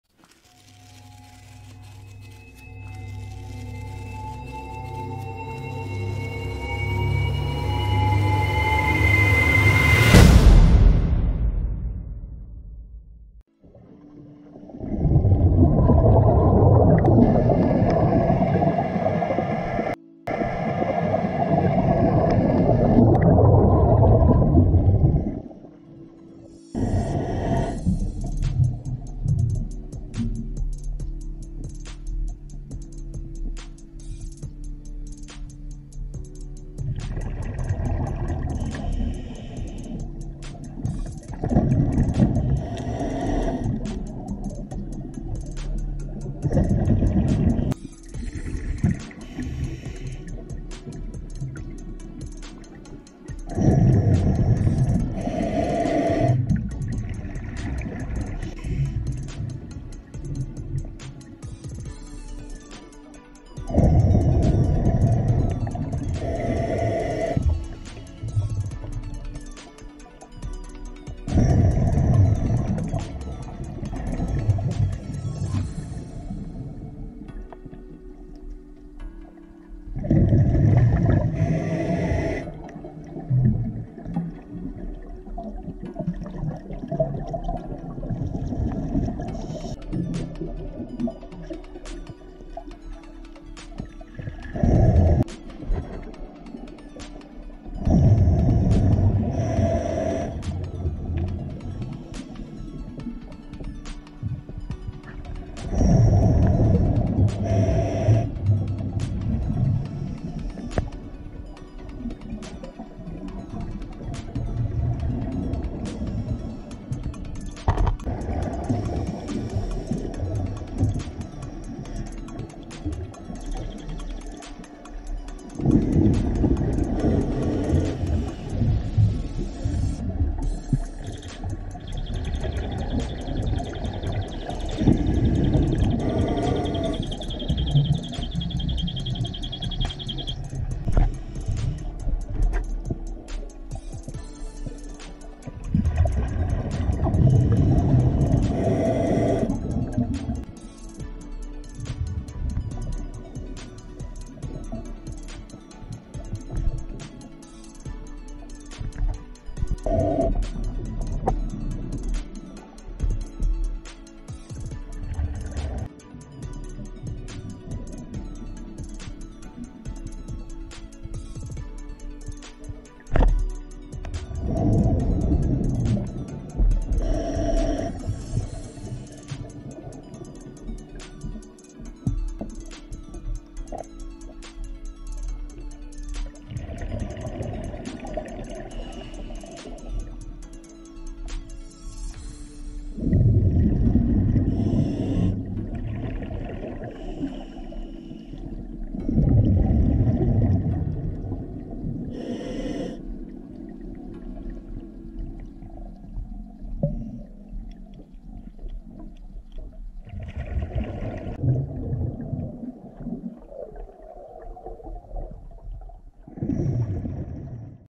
Scuba diving in Trout Lake | Boulder Junction | Wisconsin
Night Scuba Diving in Trout Lake | Boulder Junction, Wisconsin

Diving in very clear water in northern Wisconsin. The dive took place off Cathedral Point Park in Boulder Junction.

WI DNR:
Trout Lake is one of the largest lakes in Vilas County. It is one of the few inland lakes to host trout. Max depth is 117 feet, however, our deepest depth we did was 17 feet in our 2mm shorties.

scubajoe show